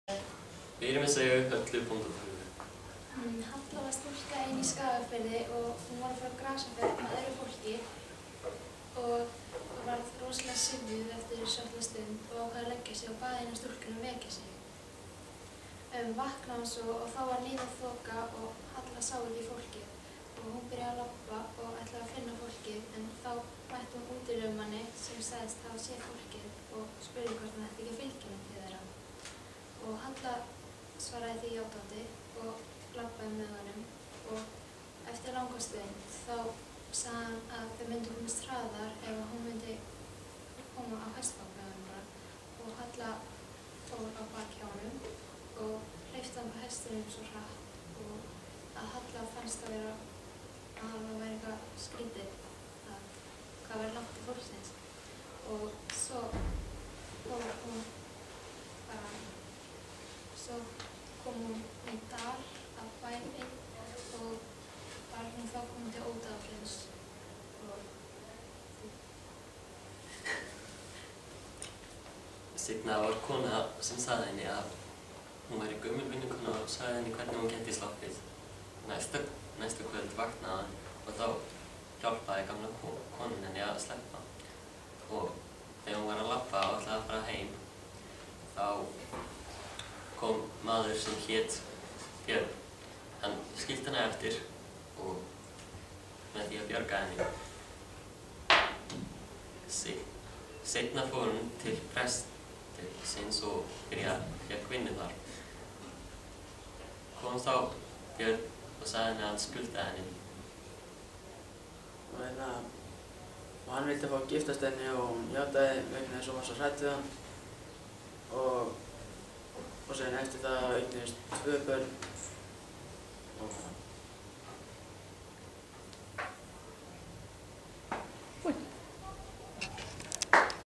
Blönduskóli, Halla bóndadóttir
Halla bóndadóttir er íslensk þjóðsaga. Hér er hún sögð af nemendum í Blönduskóla en þetta er hluti af Comeniusar verkefni (From Local Residents to European Citizens), vorið 2013.